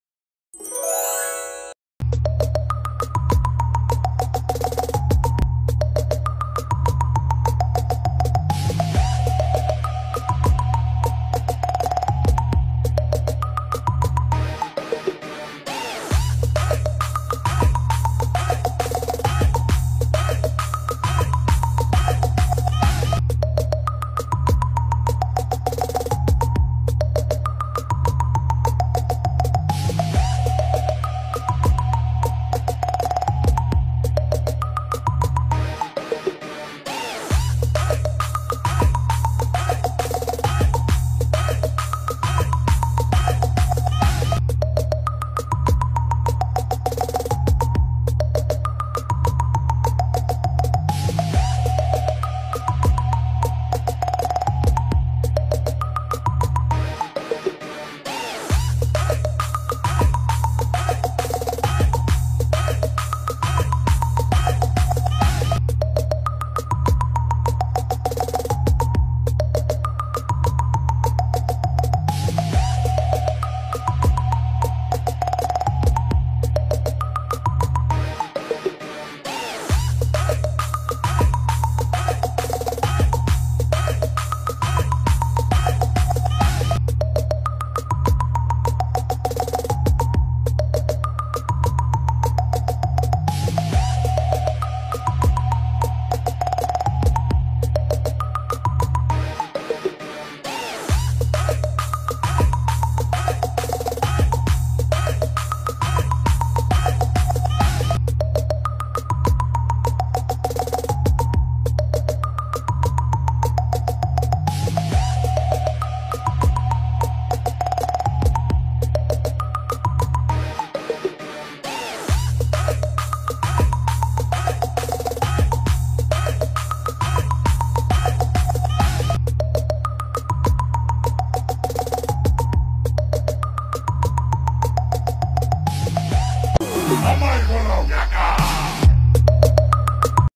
🇱🇰 කඩදාසි වලින් හදපු ගරා යකා / srilankan devil mask
srilankan devil mask

ඉවතදමන කඩදාසි වලින් හදපු අපේ රටේ ගරා යකා වෙස් මුහුණ